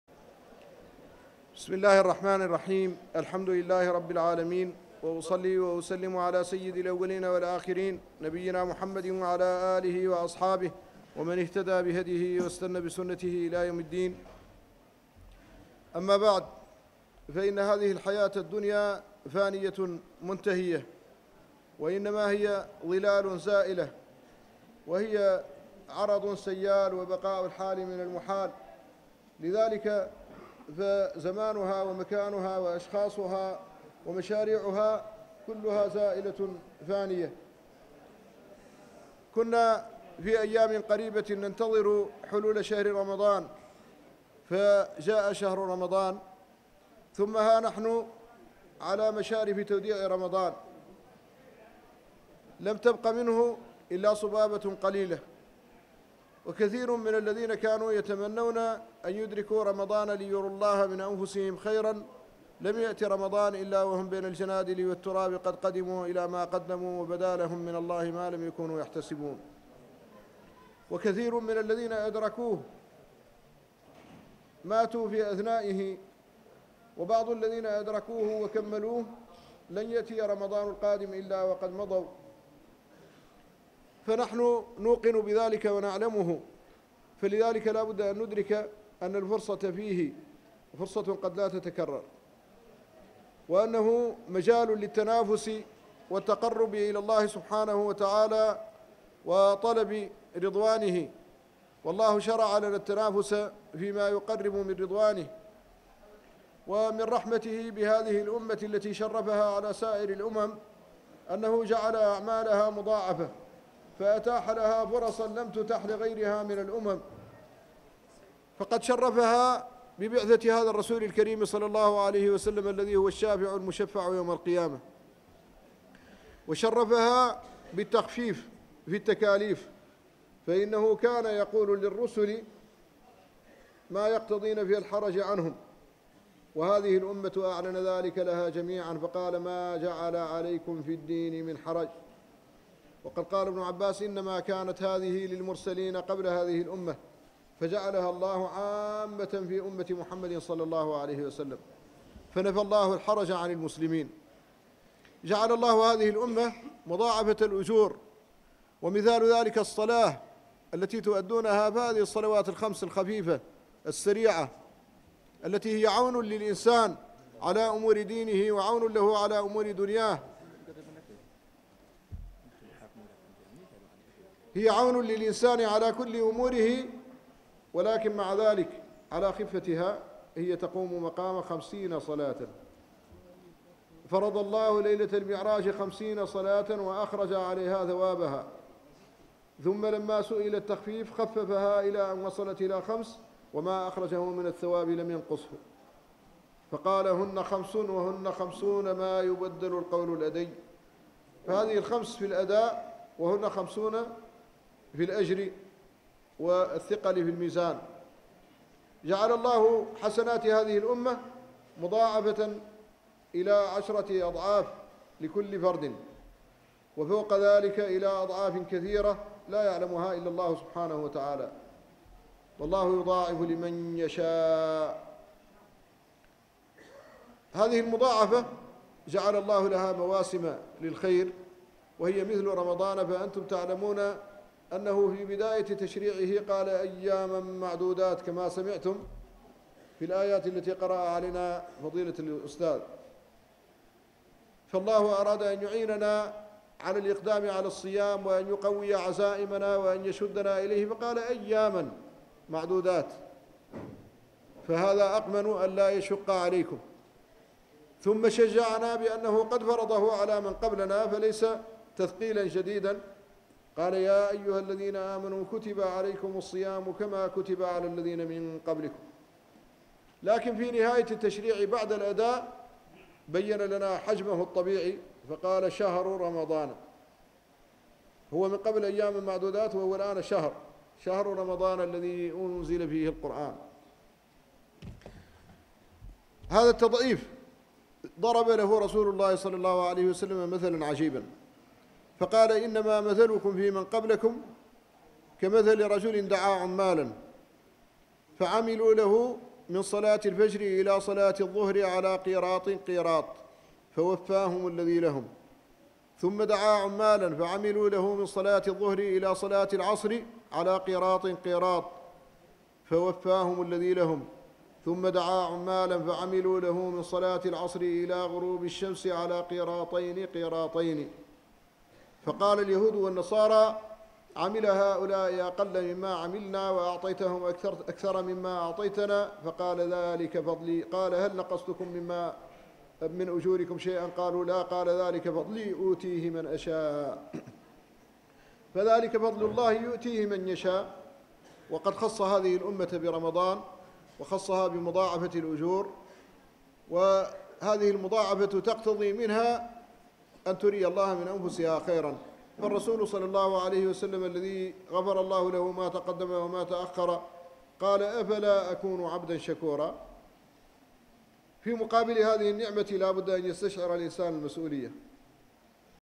مواسم الخيرات فرص قد لا تتكرر | القناة الرسمية لفضيلة الشيخ محمد الحسن الددو
اعمل الخير فربما لا تأتيك الفرصة
مواسم الخيرات فرصة قد لا تتكرر